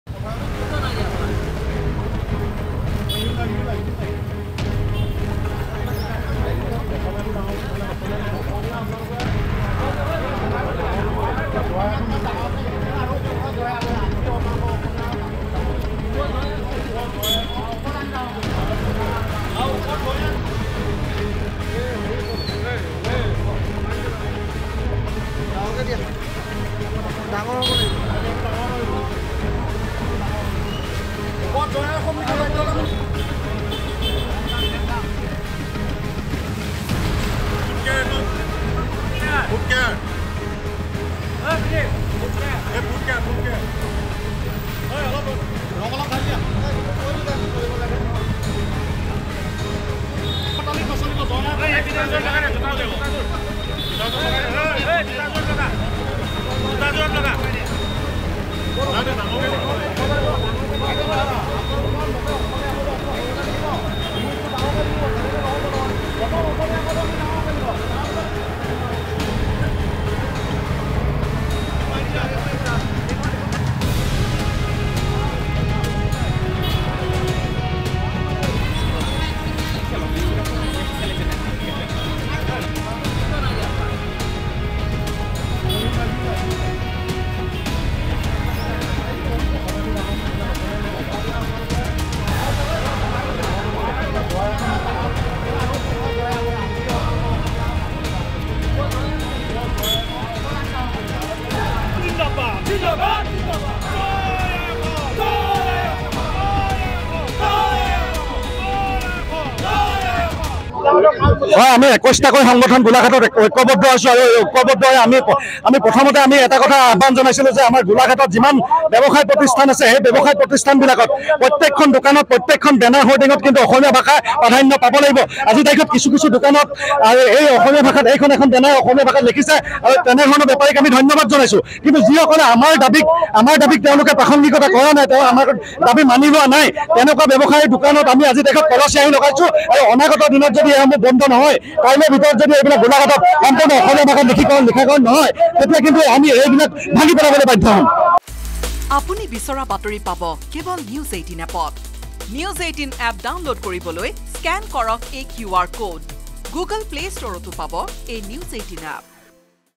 Golaghat News : ব্যৱসায়িক প্ৰতিষ্ঠানত অসমীয়া ভাষাৰ নামফলক ব্যৱহাৰ কৰাৰ দাবীত প্ৰতিবাদ | N18V
Golaghat News |  গোলাঘাটৰ প্ৰতিখন ব্যৱসায়িক প্ৰতিষ্ঠানত অসমীয়া ভাষাৰ নামফলক ব্যৱহাৰ কৰাৰ দাবীত প্ৰতিবাদ ৷ গোলাঘাট নগৰৰ মাজমজিয়াত থকা যি সমূহ ব্যৱসায়িক প্ৰতিষ্ঠানৰ নামফলকত অসমীয়া ভাষাৰ পৰিৱৰ্তে অন্য ভাষা ব্যৱহাৰ কৰিছে তাত ক’লা ৰং সানি প্ৰতিবাদ সাব্যস্ত কৰে ২১ টাকৈ জাতীয় দল সংগঠনে৷